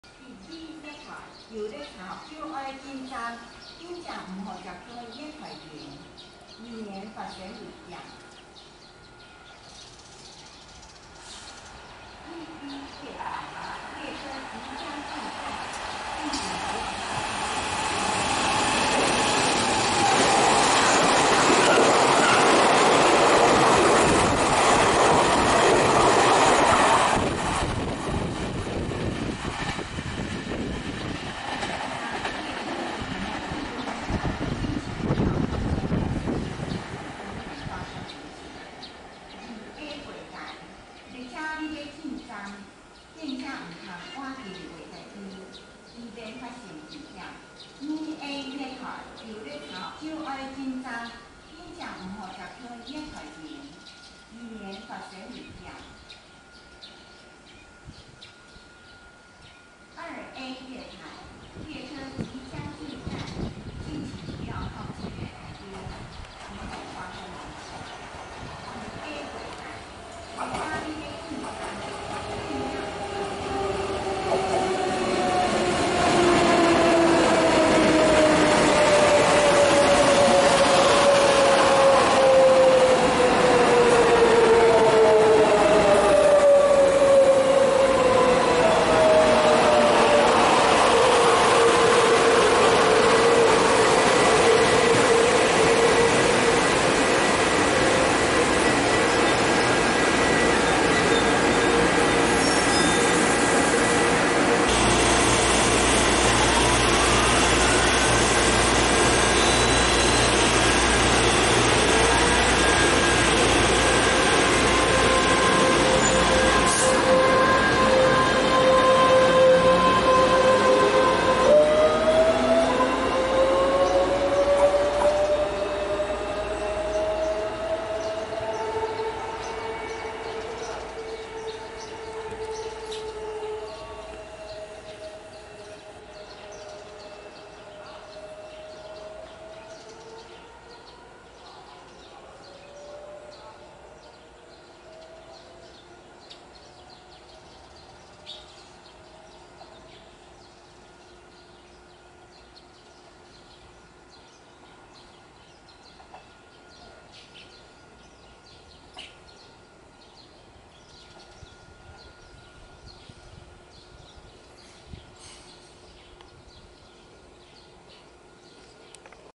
2019-07-29 中洲車站莒光號(本務E223)1B通過及EMU500(534+573)2A進出站
莒光號     513車次  海  七堵開往潮州
區間車  3238車次  山  潮州開往后里